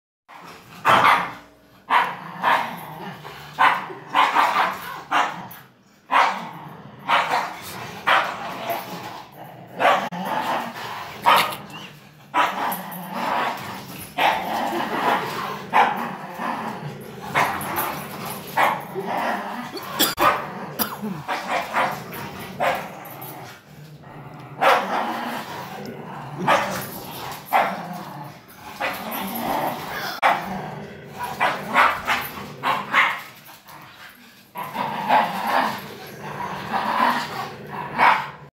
German Shepherd vs Min Pin
Tug of war between our 9 month old German Shepherd and our 5 year old Min Pin.